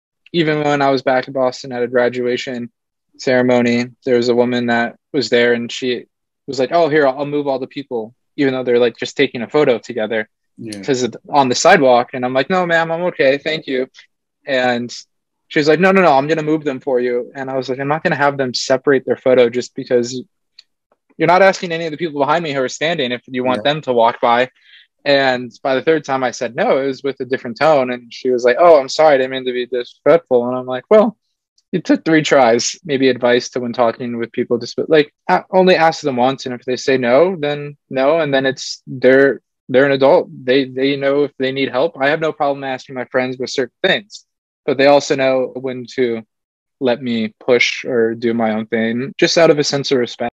How to interact with people with disabilities
In this clip, I ask Kainoa, if there’s anything he’d like to give advice to people without disabilities on treating people with disabilities?

He says, only ask once if they need help, treat them with respect and trust them as an adult.
Kainoa Spenser is a necrotizing fasciitis (flesh-eating bacteria) survivor and a quadruple amputee and will speak about his life experience.